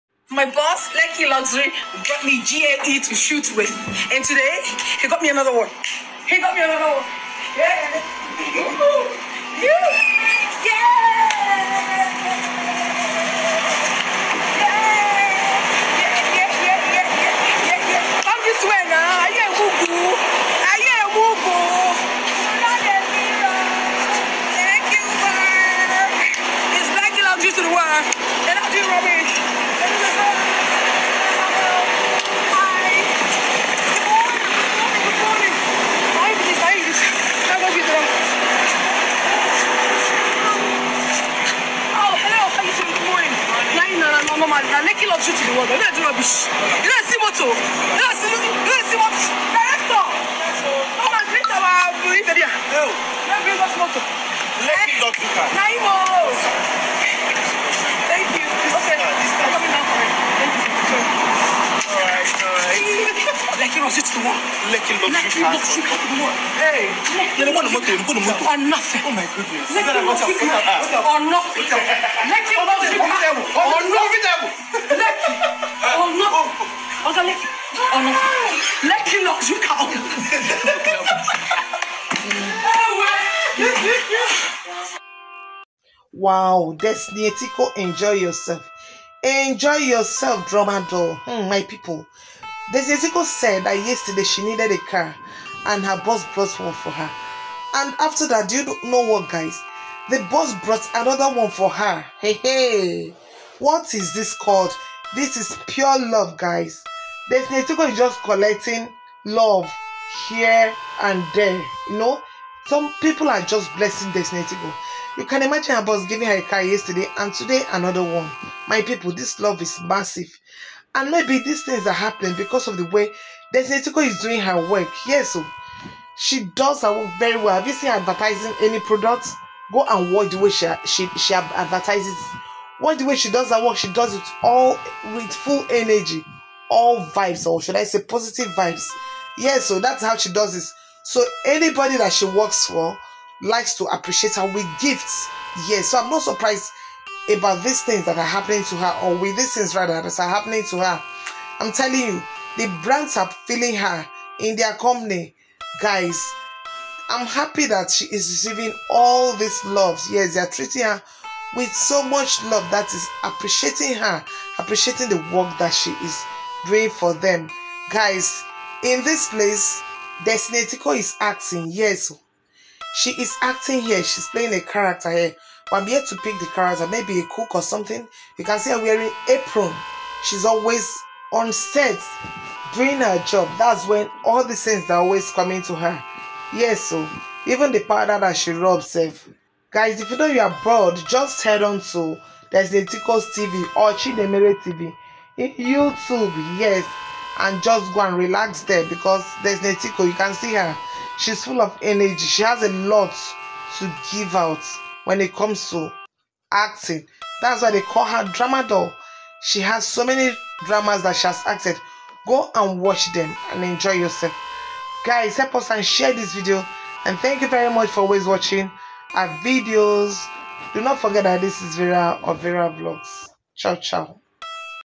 The moment Destiny Etiko received luxurious cars from her boss. Drama doll is really loved.
The moment Destiny Etiko received luxurious cars from her boss. Drama doll is really loved by many. This is because Destiny Etiko shows love to many people as well.